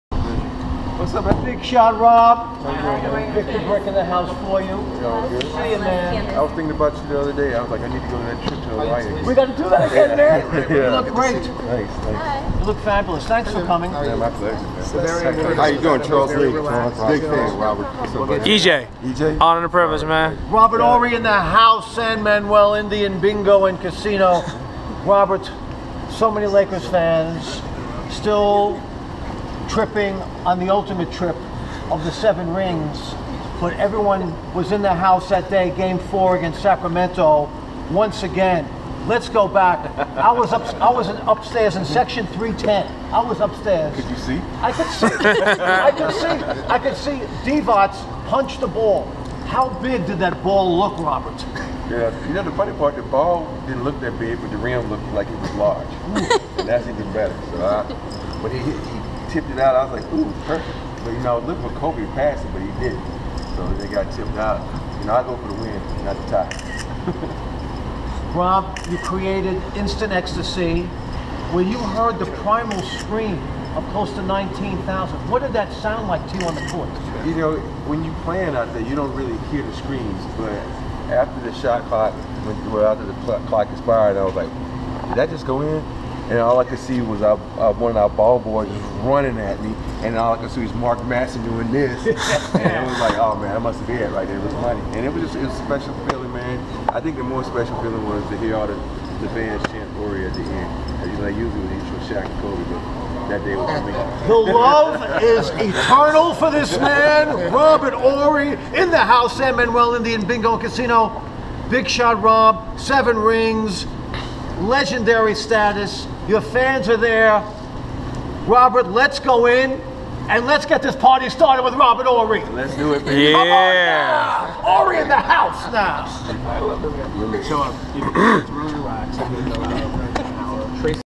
LAKERS HAPPENING @ SAN MANUEL CASINO WITH ROBERT HORRY
LAKERS FANS

FEELIN YOU

LAKERS HAPPENING @ SAN MANUEL INDIAN BINGO AND CASINO

LEGEND IN THE HOUSE

BIG SHOT ROB THROWIN DOWN

ROBERT HORRY LOVE FEST IN FULL EFFECT

THE SHOT STILL RESONATES FROM MAY 26TH 2002

DOWNTOWN HOOP DOJO

SACRAMENTO-LOS ANGLES WESTERN FINALS

KINGS ON CUSP OF SNATCHING A MAS FUERTE 3-1 LEAD GOING BACK TO THE THEN ARCO-ARENA

THE COWBWLL-HELL

SUDDENLY ROBERT HORRY

GREATEST SHOT IN LAKERS TIME AND SPACE

EARNING HORRY

BIG SHOT ROB IN LAKER LORE..

LET THE HORRY SHOT RUN AMOK THRU YOUR SOUL...

THE PRIMAL SCREAM

WE CAN ALL STILL HEAR IT

I SCMOOZED BIG SHOT ROB AS HE ARRIVED AT SAN MANUEL